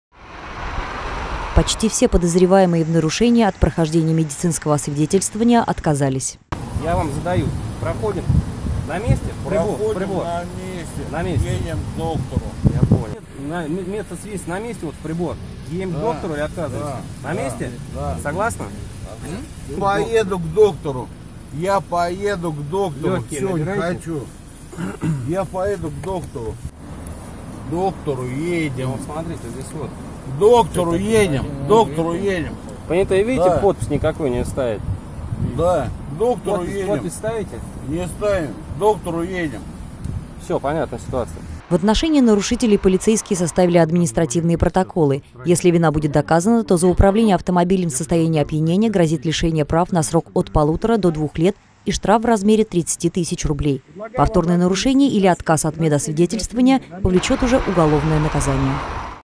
В Волгограде прошли рейды ГАИ
Лыка не вяжет, но за рулем – в последние выходные дни новогодних праздников волгоградские полицейские остановили на дорогах 18 водителей с признаками опьянения.